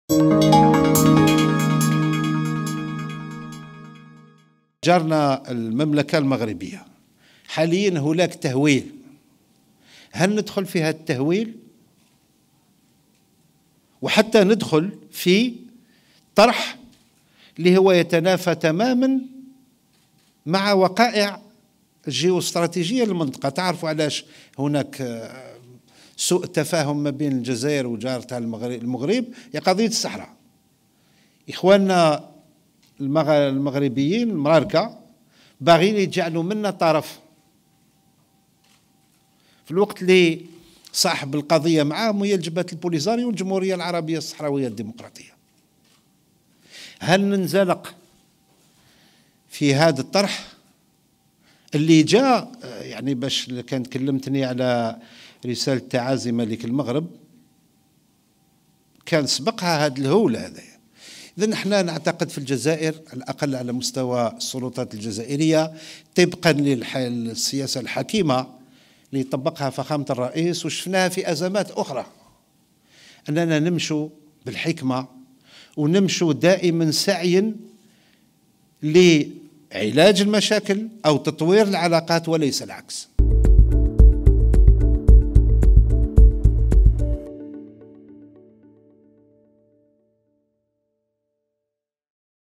أويحيى: الجزائر لن تنزلق وراء حملة "التهويل" و تستخدم "الحكمة" في معالجة الأزمات
الجزائر (وأج)- أكد الوزير الأول أحمد أويحيى في ندوة صحفية أنّ الجزائر لن تنزلق وراء حملة "التهويل" و تستخدم "الحكمة" في معالجة الأزمات.